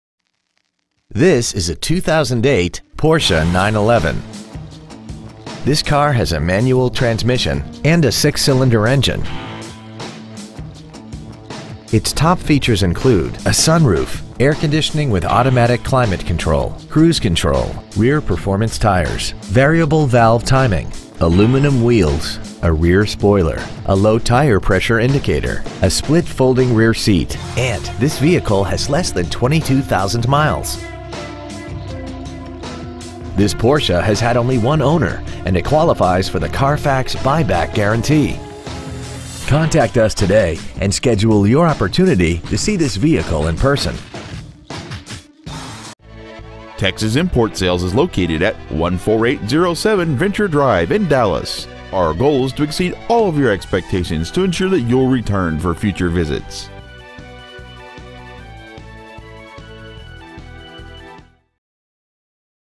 Preowned 2008 Porsche 911 Dallas TX
Year : 2008
 Make : Porsche
 Model : 911
 Engine : 3.8L horizontally-opposed DOHC 24V 6-cyl engine
 Trans . : Manual
 Exterior : Black
 Miles : 21,267
 Interior : Black
 Stock : 2986

 Used 2008 Porsche 911 Dallas TX